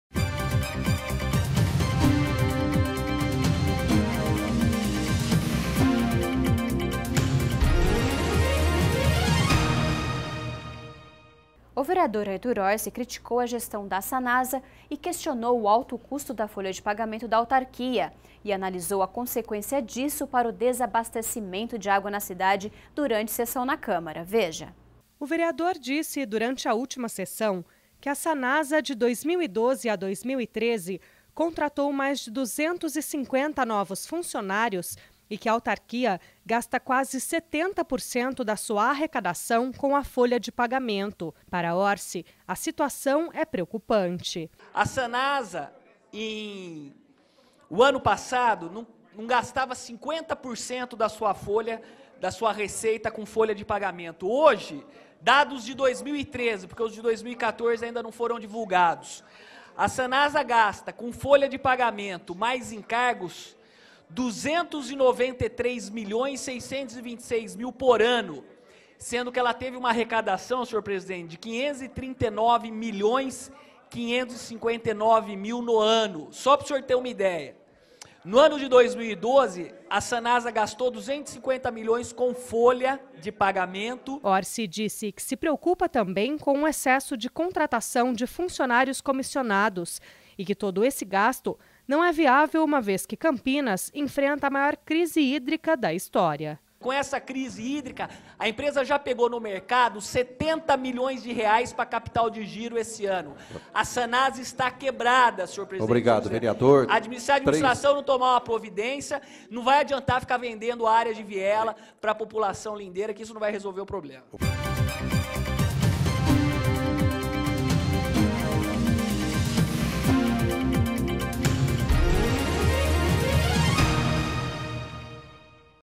CÂMARA EM NOTÍCIA : ORSI CRITICA GESTÃO DA SANASA 22/10/2014
O VEREADOR ARTUR ORSI (PSDB) CRITICOU A GESTÃO DA SANASA, QUESTIONOU O ALTO CUSTO DA FOLHA DE PAGAMENTO DA AUTARQUIA E ANALISOU A CONSEQUÊNCIA DISSO PARA O DESABASTECIMENTO DE ÁGUA NA CIDADE DURANTE SESSÃO DA CÂMARA.